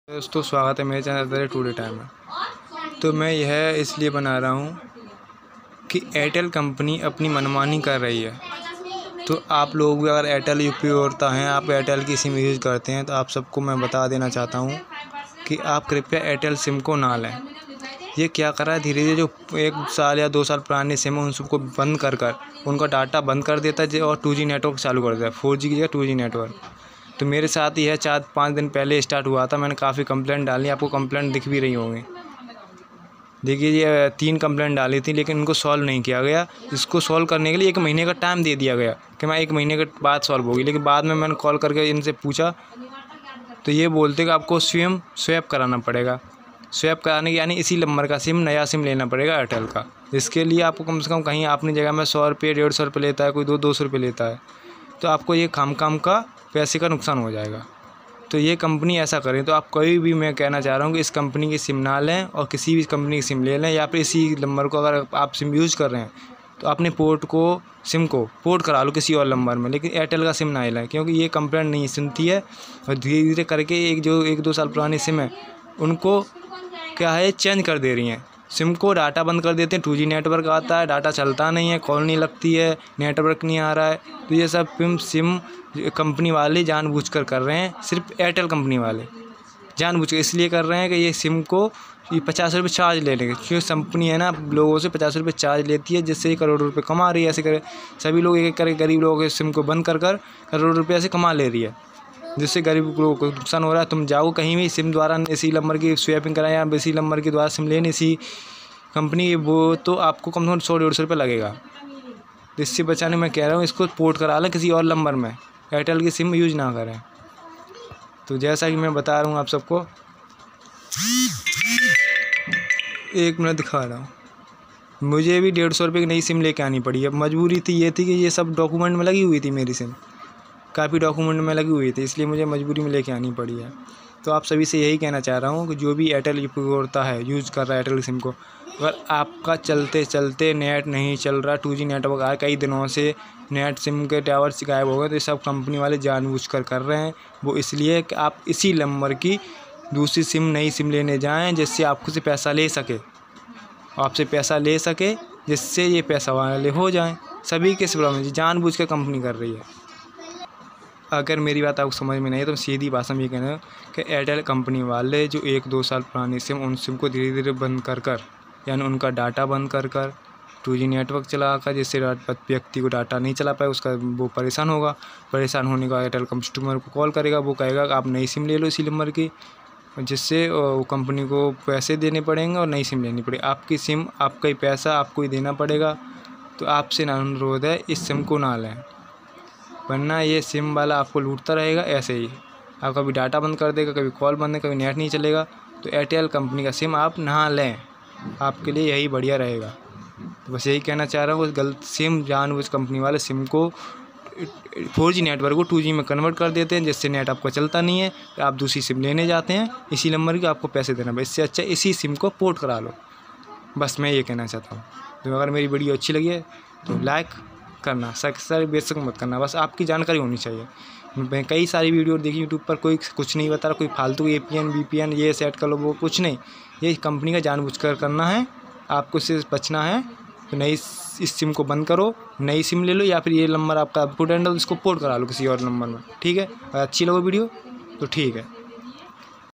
एयरटेल की सिम में 4G की है 2G नेटवर्क हो गया है बहुत बड़ी समस्या क्या है वीडियो को पूरा देखे
एयरटेल कंपनी द्वारा एयरटेल के कस्टमर को बहुत ज्यादा परेशान किया जा रहा है जिस कारण कभी नेटवर्क यीशु कभी कॉल यीशु कभी s.m.s. यीशु की सब परेशानियां झेलनी पड़ रही है 4G डाटा कभी 2G में बदल जाता है 4G डाटा 2G में बदलने के कारण कंपनी जानबूझकर कर रही है कंपनी लोगों साथ इस तरह फ्रॉड किया जा रहा है कि वह कम लोगों की सिम को बंद कर उसी नंबर का नया सिम लेने का अनुरोध करते हैं जिससे उनका कंपनी को फायदा होता है
मैं यह कहना चाहता हूं एयरटेल कंपनी हम लोगों को चूना लगा रही है और अपना कमाई कर रही है लोगों को इंटरनेट को बंद करके लोगों को परेशान कर रही है 4G के 2G नेटवर्क कर रही है जिससे उन्हें उसी नंबर की नई सिम एयरटेल कंपनी की लेनी पड़ती है जिससे एयरटेल उपभोक्ता को कंपनी को नई सिम के पैसे देने पड़ते हैं है जो कंपनी लेती है यह परेशानी यूट्यूब पर कोई नहीं बता रहा है
AIRTEL 4G TO 2G PROBLEM
airtel Sim problems